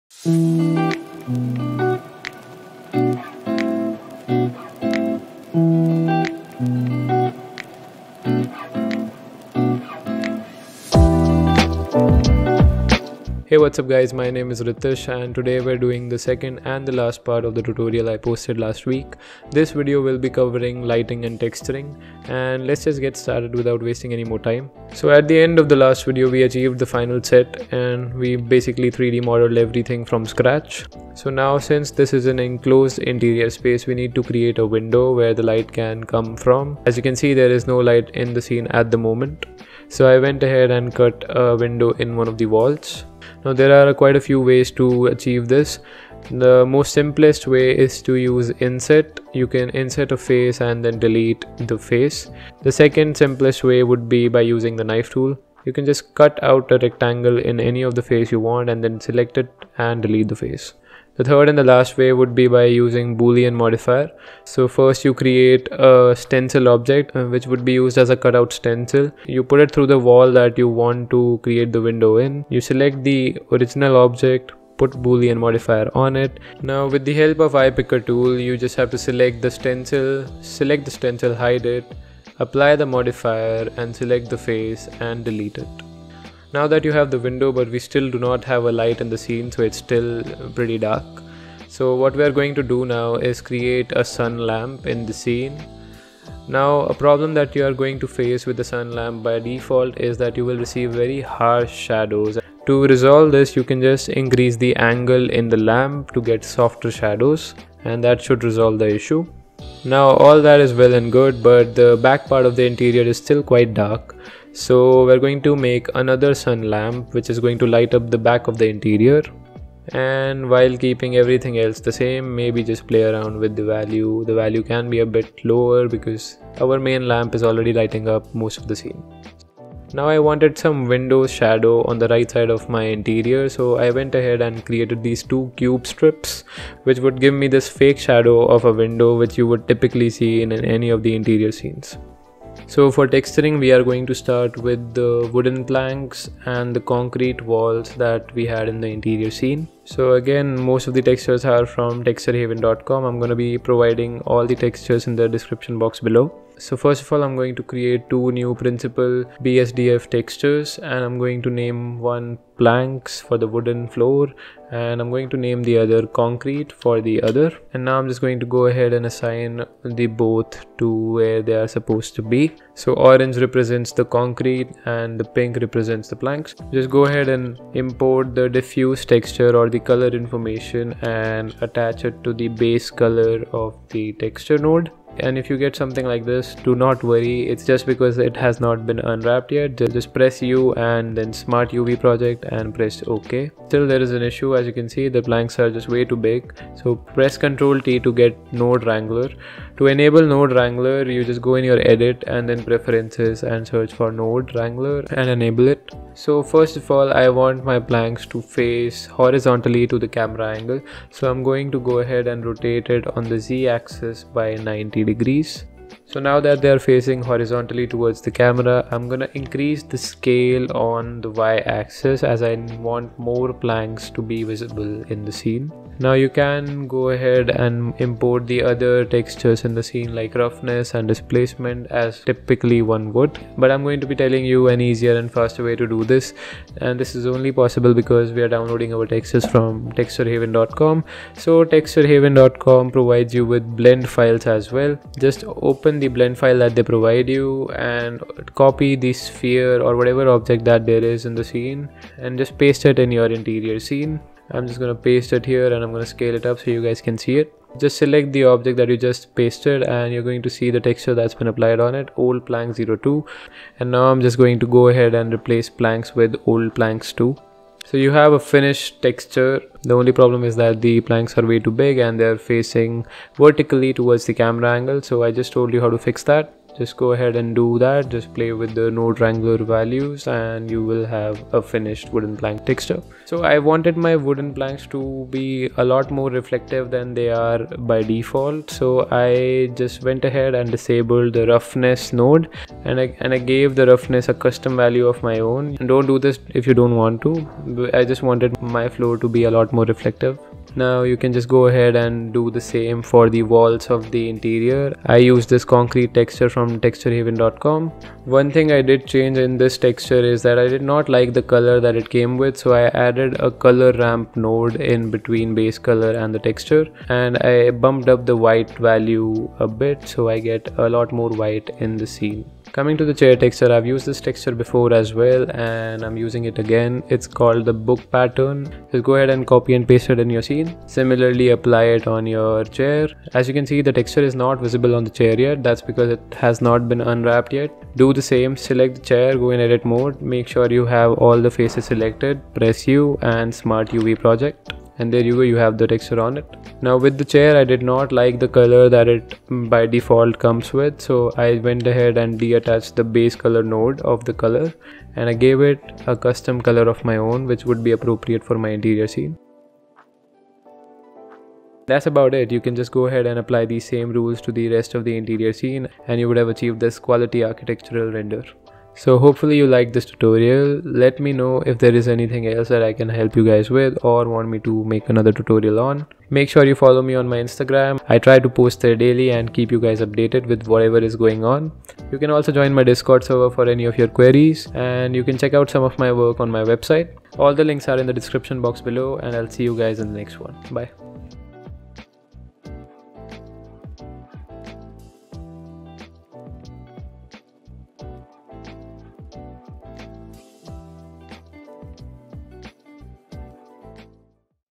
Blender Step-by-Step Create a realistic Interior (2. Lighting and texturing)
Introduction  0:00 - 0:29
Lighting         0:30 - 2:22
Texturing      2:23 - 6:00
Outro             6:00 - 6:45

Hey there, my name is Ritish Suneja and this video is a sequel following my previous upload. In this video I cover texturing and lighting of the scene.

White marble  - shorturl.at/gvCO1

All the other textures used are simple Principal BSDF textures with different properties like metalic, reflective and vice versa.